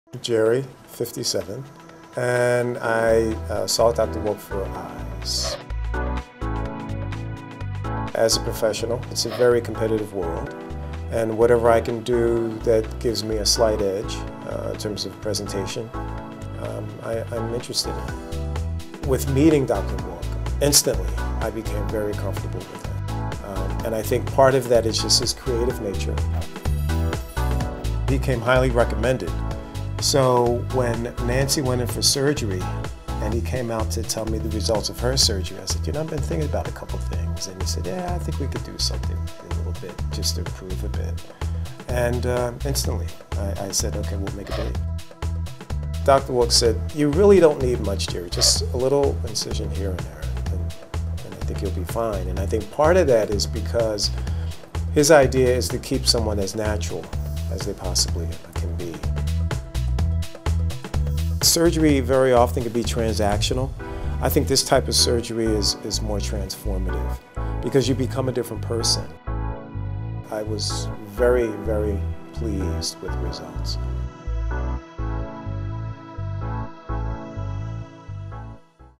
Eye Surgery Testimonial at W Cosmetic Surgery: Jerry
Jerry is a 57-year-old man who came into Dr. Wulc's office for his eyes. Jerry works in an extremely competitive field and was looking for a way to improve his overall presentation. He noticed Dr. Wulc's creative nature which made him feel more comfortable with the procedure.

Jerry's experience highlights one of the most important goals we have at W. Cosmetic Surgery, to keep as natural of a look as possible. For jerry, that meant there were just a couple of incisions needed to get him where he wanted to be but still appear to be completely natural.